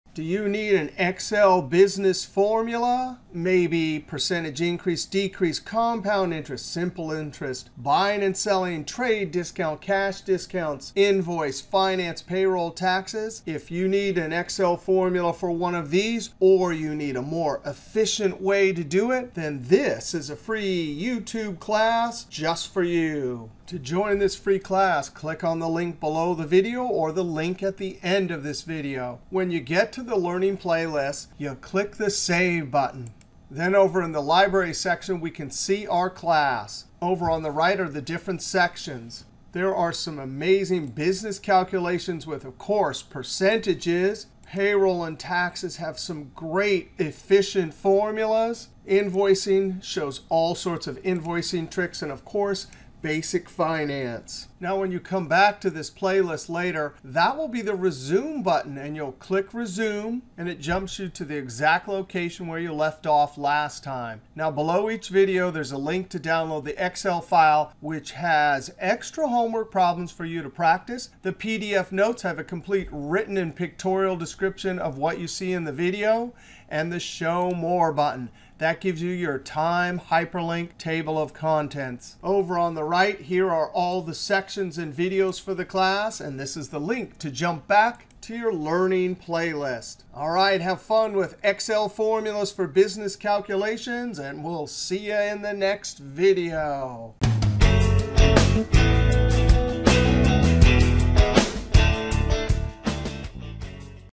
Free Excel Business Math & Formulas Course at YouTube
Some of the topics:
1. % Increase Decrease Problems
2. Compound Interest
3. Simple Interest
4. Buying & Selling
5. Trade Discounts
6. Cash Discounts
7. Invoicing
8. Finance
9. Payroll
10. Taxes
11. VLOOKUP & IF
12. Much More…